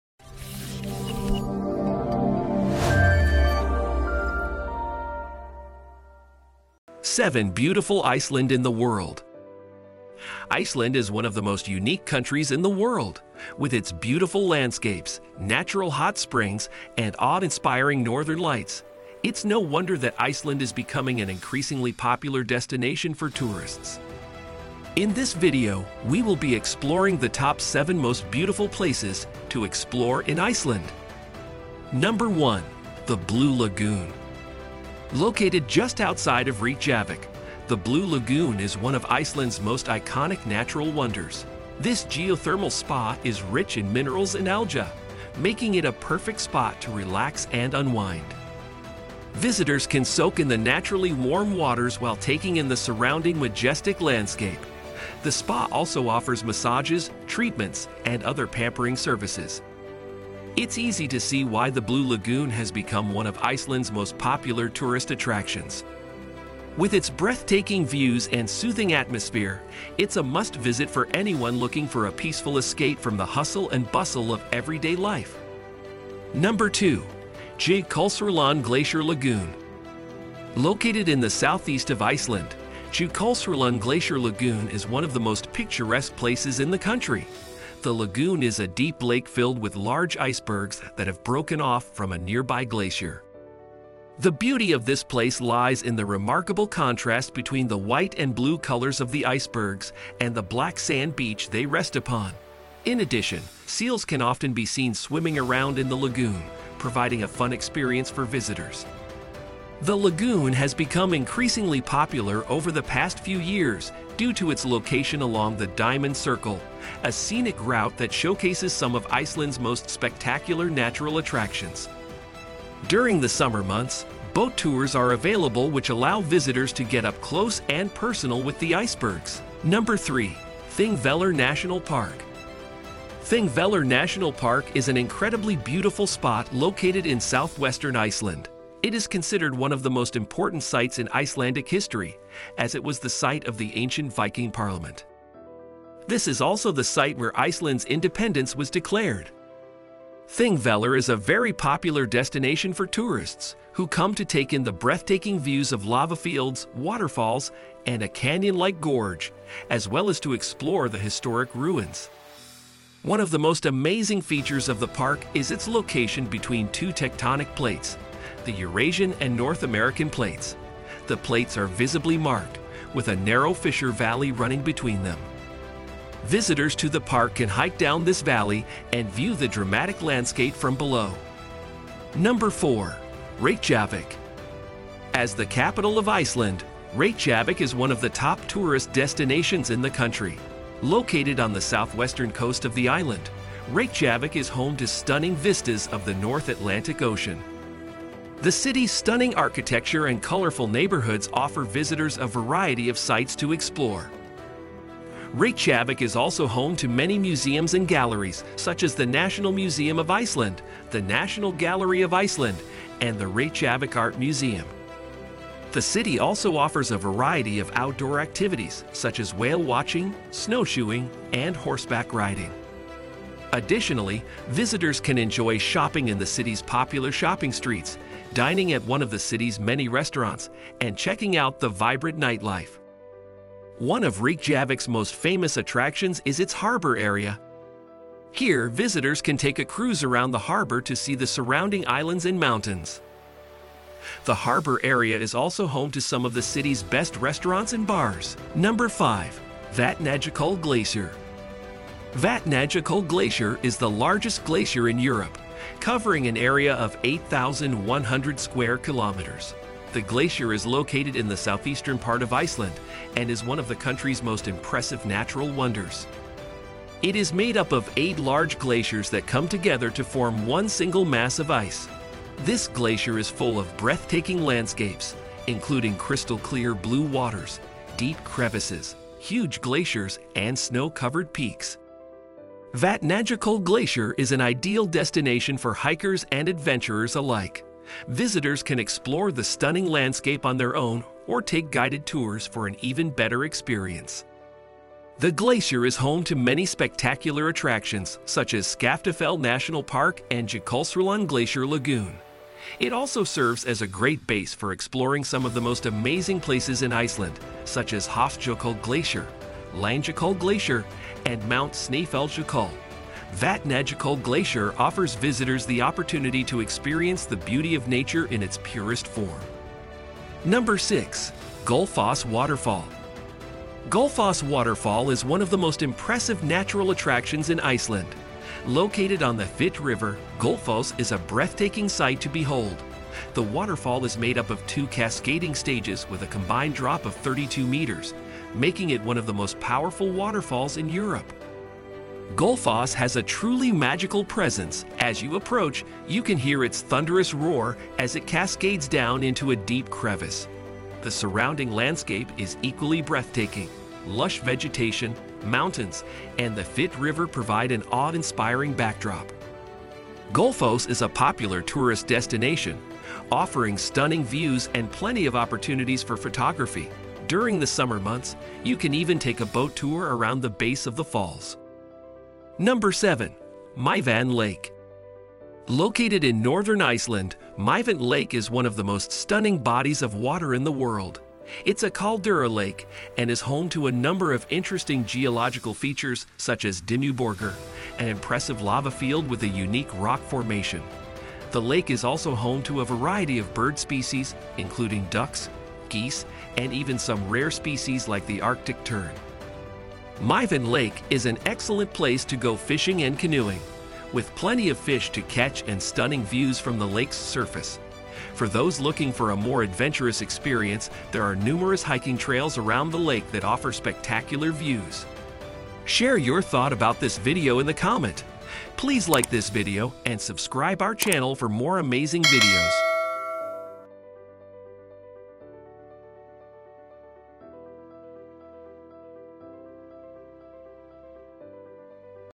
7 beautiful Iceland in the world
In this video we will discuss 7 beautiful Iceland in the world Iceland is one of the most unique countries in the world. With its beautiful landscapes, natural hot springs, and awe-inspiring Northern Lights, it's no wonder that Iceland is becoming an increasingly popular destination for tourists.

Testimonytees.org